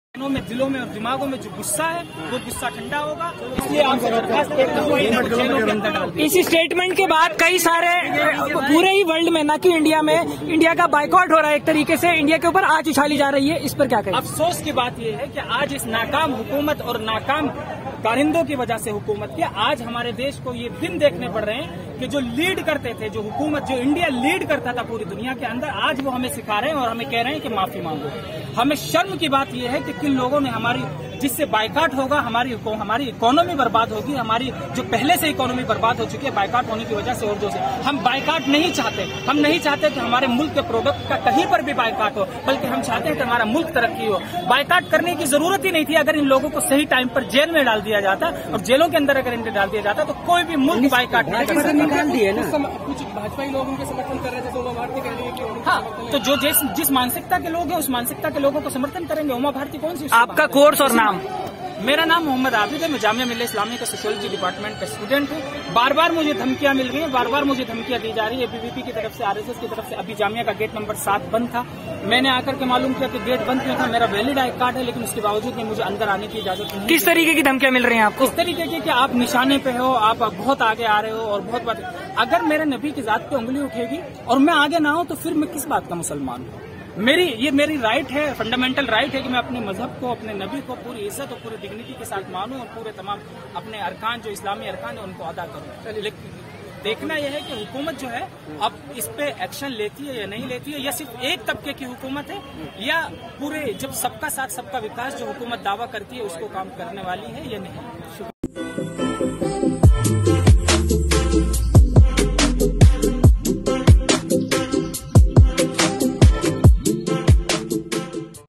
Protest Against Ex-BJP Spokesperson Nupur Sharma at JMI
Student Members of Campus Front of India, Jamia Millia Islamia protested against Ex-BJP spokesperson Nupur Sharma and demanded her arrest.

TJR interacted with one of the students at the campus, to learn more about the reason behind the protests.

Credits: Arsalan Shamsi